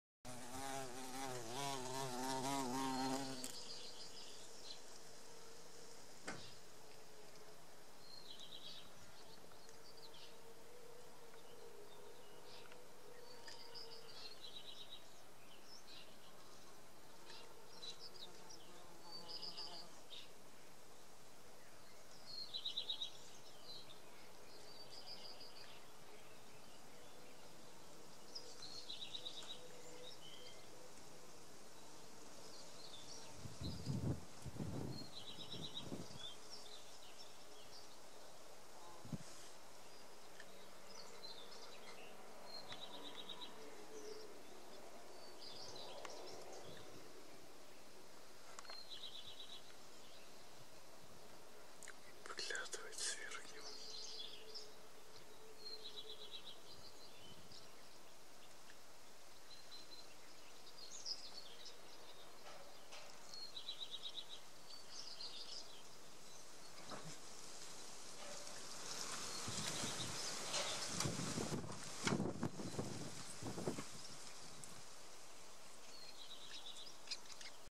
Пчела-плотник.
Май 2018 года, Самарская область. Заметив крупных черных летающих насекомых в саду, начал искать их место жительства. Поиски увенчались успехом - знакомьтесь!